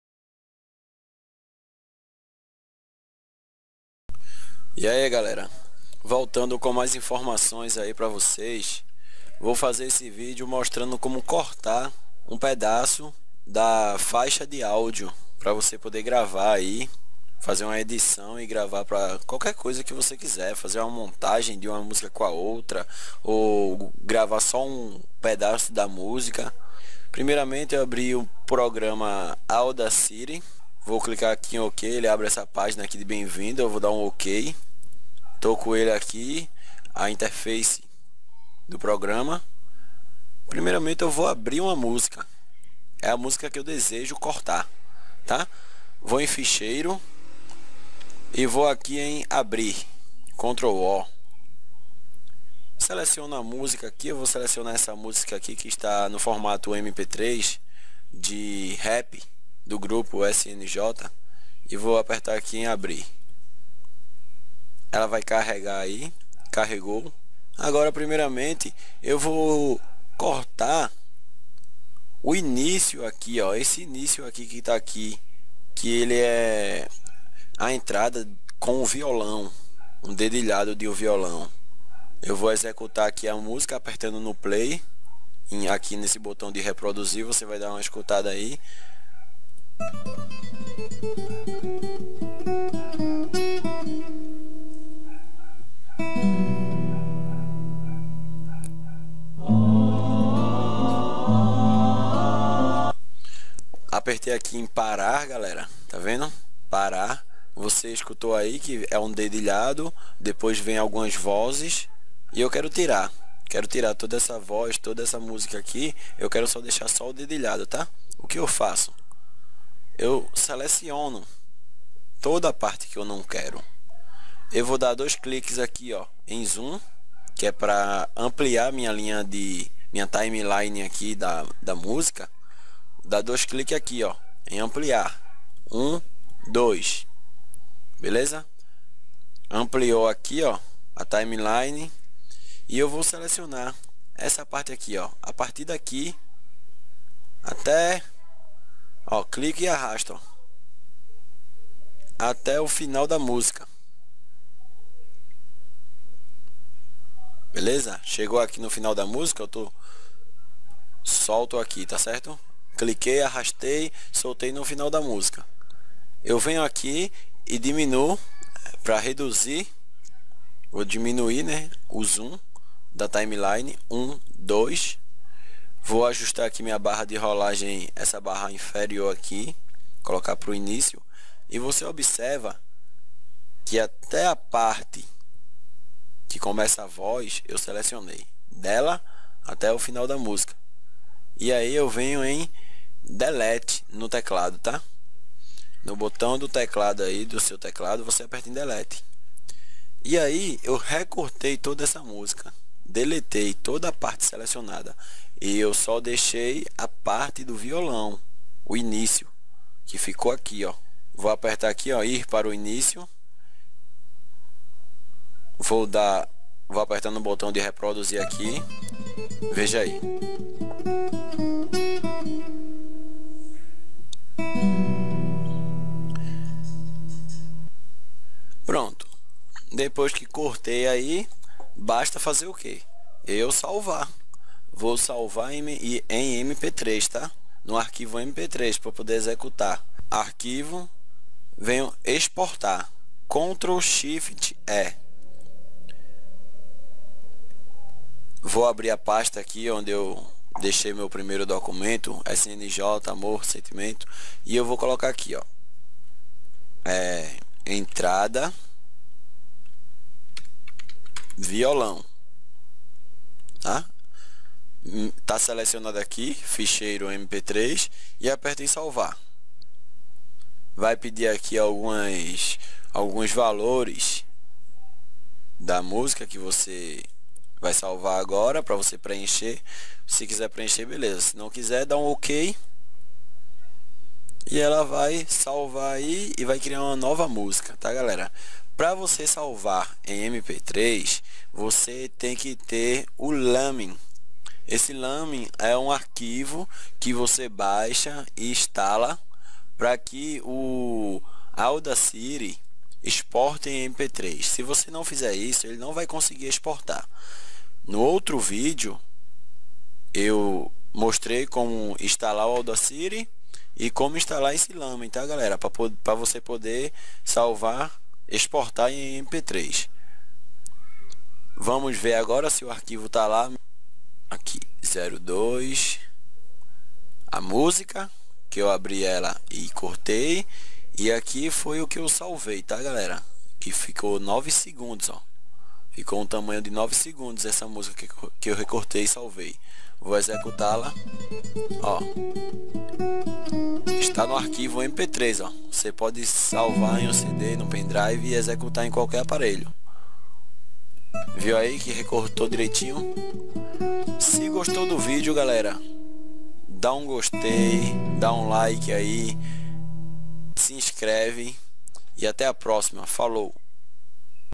Como cortar musica MP3 - Audacity
Segue esse vídeo para a demostração de uma das maneiras de como cortar uma faixa de música usando o programa Audacity. Simples e fácil para você que está precisa fazer uma edição de sua música ou até mesmo retirar aquele pedacinho do áudio que você gravou.

Assista e confira!